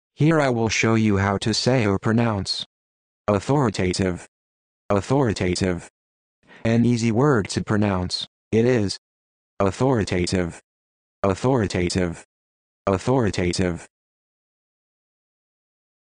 How to Say or Pronounce Authoritative
This video shows you how to say or pronounce Authoritative.

How accurate does it say Authoritative? How would you say Authoritative?